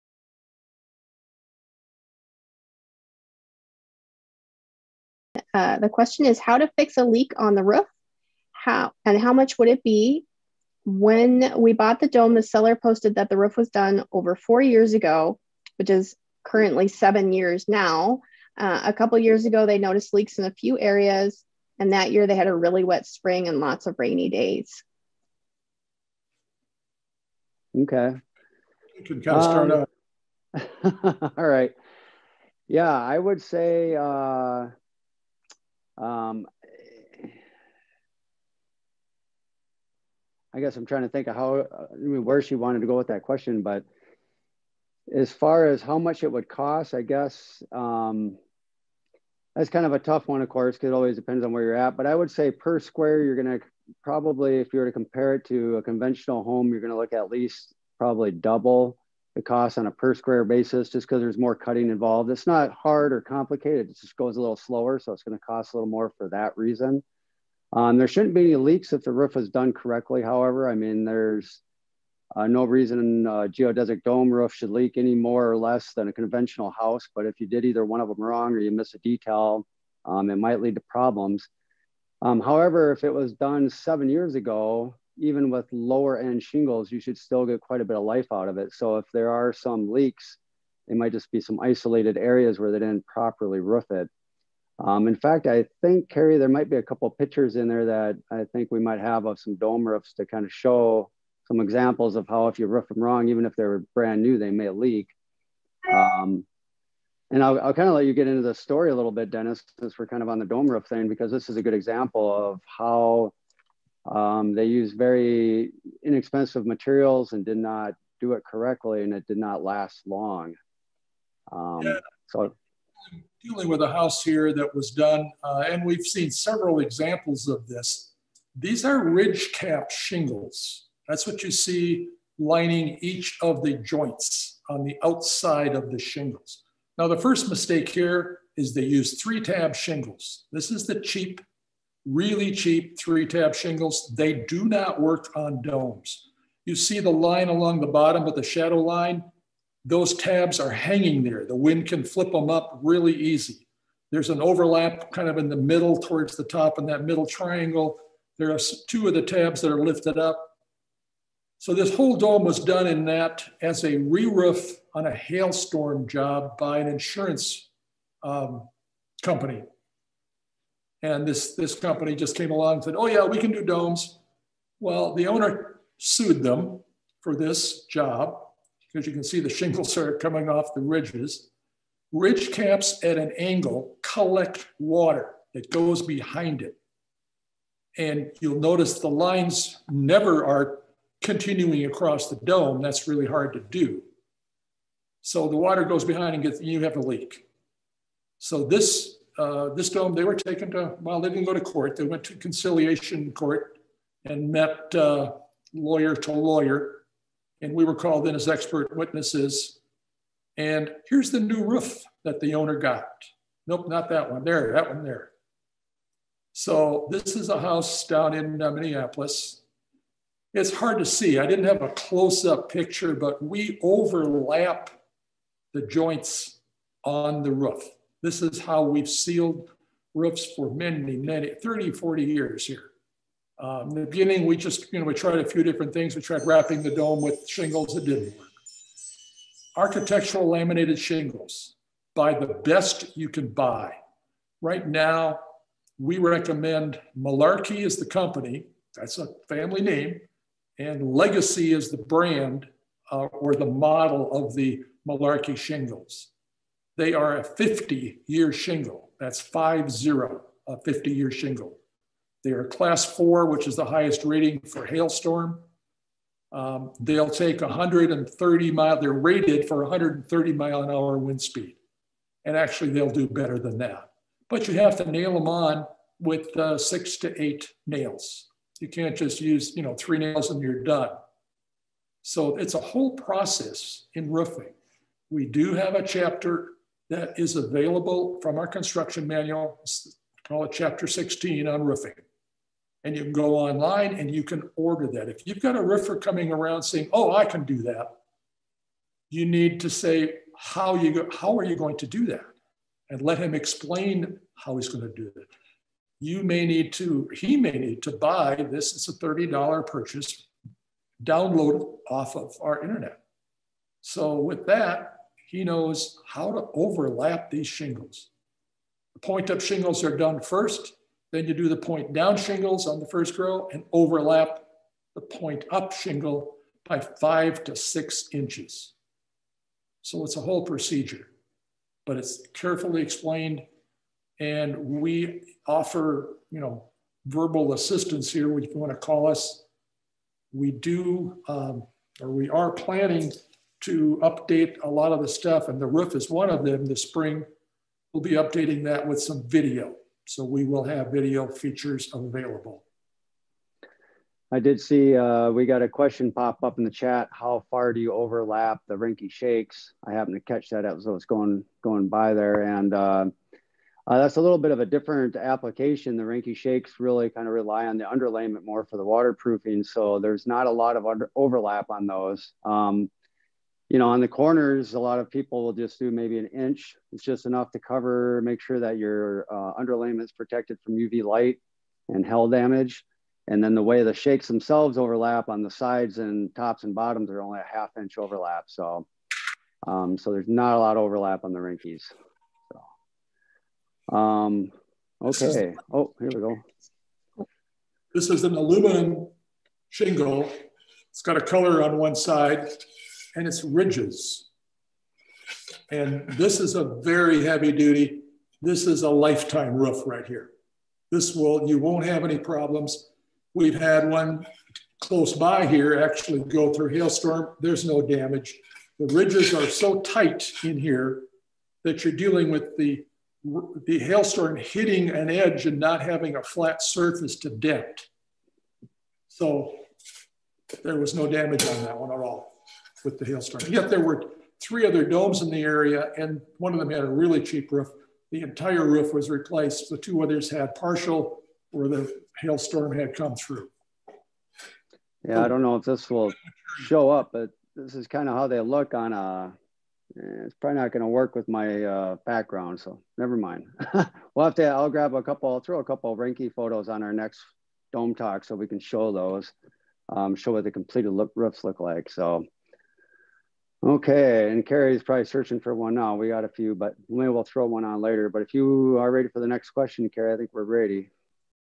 How to fix a leak on the roof and how much would this cost?
to learn more about domes or inquire about a dome.